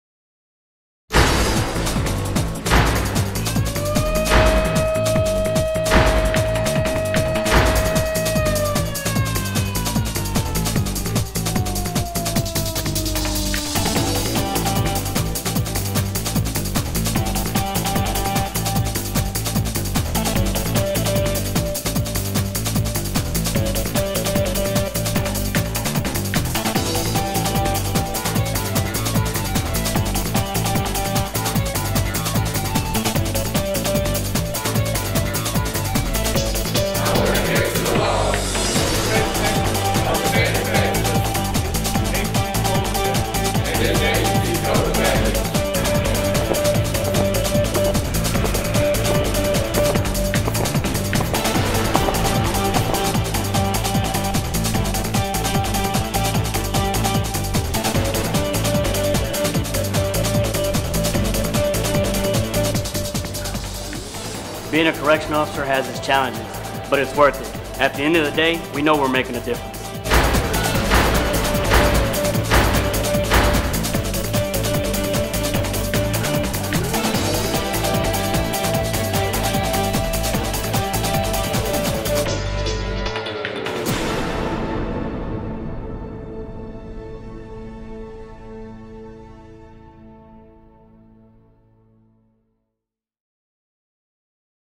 TDCJ Correctional Officer Recruitment
Courage. Commitment. Perseverance. Integrity. Can you meet the challenge? Learn how to become a professional correctional officer at  the TDCJ.